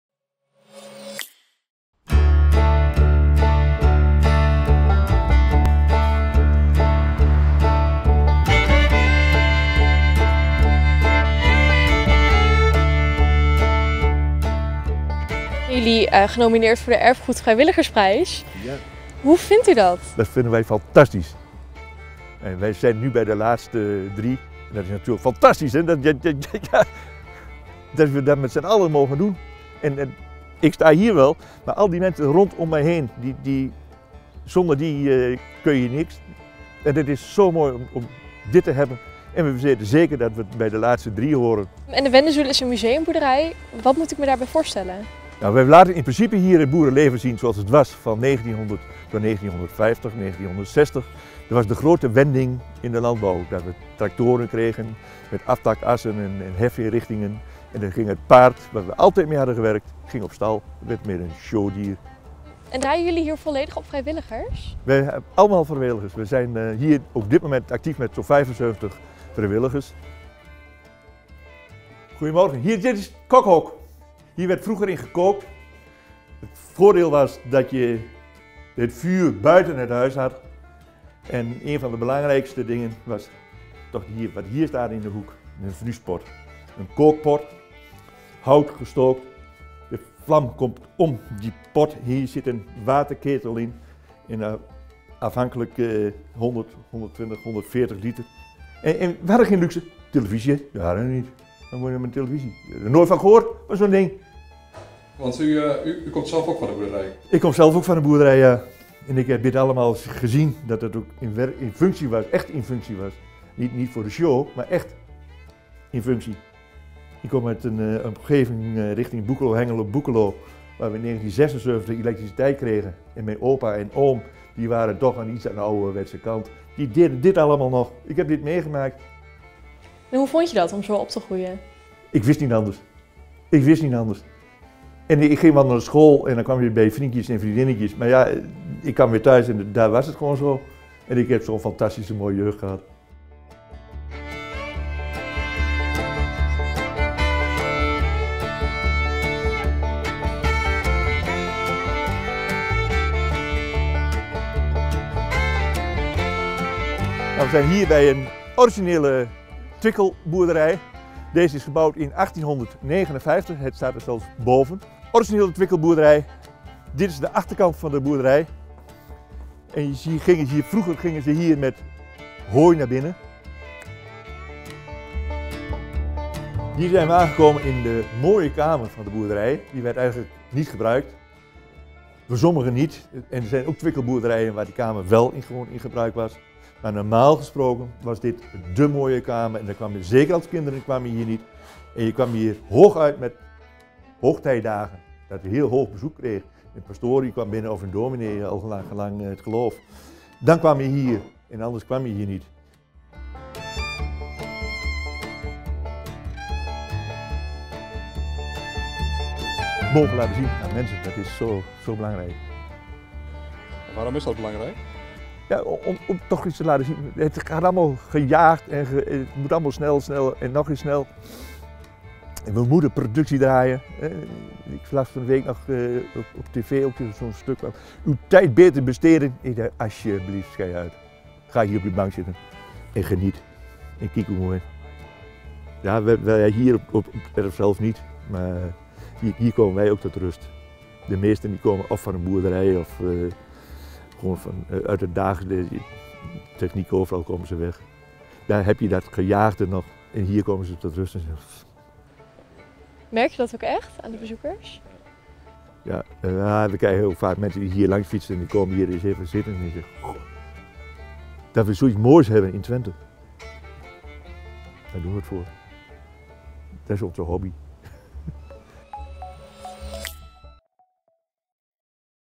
Museumboerderij Wendezoele genomineerd voor Erfgoedvrijwilligersprijs
De vrijwilligers van museumboerderij de Wendezoele in Delden zijn genomineerd voor de landelijke Erfgoedvrijwilligersprijs. De ruim zeventig vrijwilligers onderhouden het historische Erve Brinkcate op landgoed Twickel en geven bezoekers een kijkje in het Twentse boerenleven van circa 1900 tot 1960. “Zonder alle vrijwilligers zou dit erf niet meer bestaan”, zegt vrijwilliger Dirk Haan.